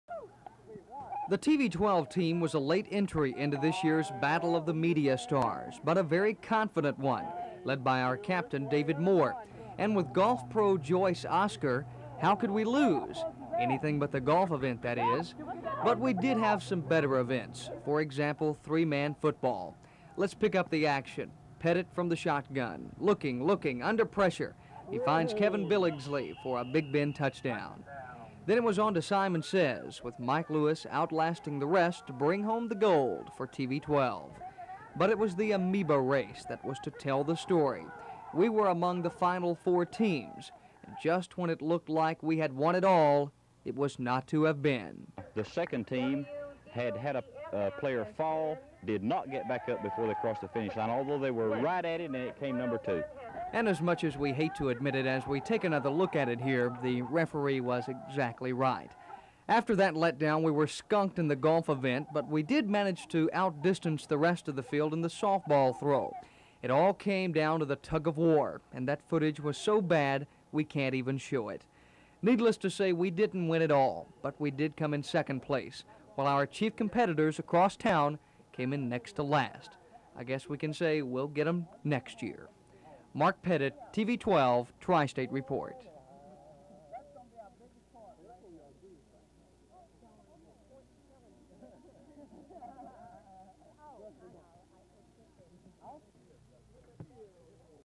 From the Archives: Media Competition in 1984
This story originally aired on WDEF TV in May of 1984.